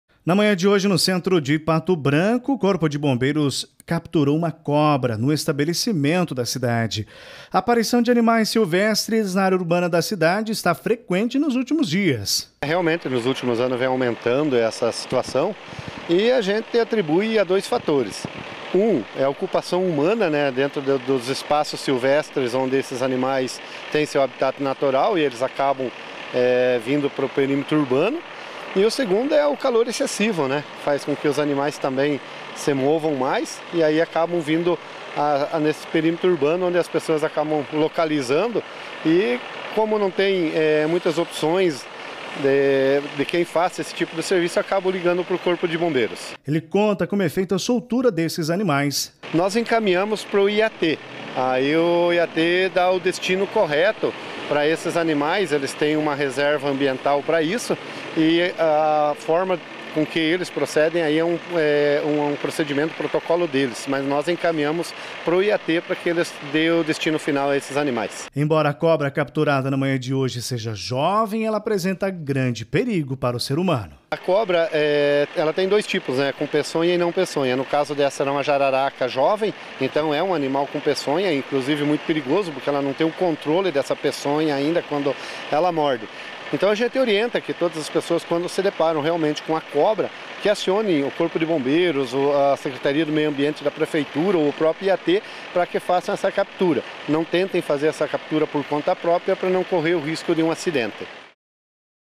Corpo de Bombeiros capturaram uma cobra na manhã de hoje (15)
Segurança: Com aumento da temperatura virou quase rotina esse trabalho dos bombeiros.